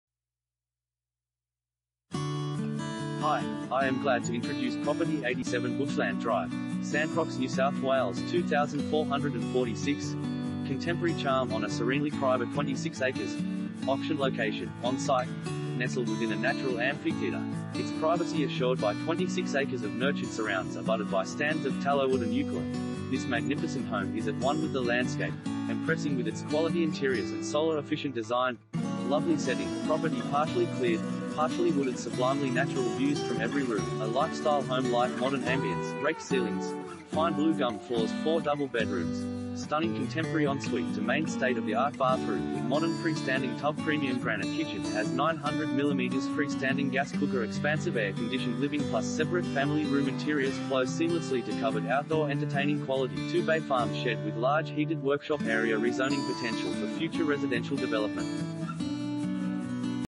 87 Bushland Drive, Sancrox NSW 2446 | AuPropertyReport.Com
87 Bushland Drive, Sancrox NSW 2446
Property Type: house
Bedrooms: 4
Bathrooms: 4
Car Space: 6
Contemporary charm on a serenely private 26 acres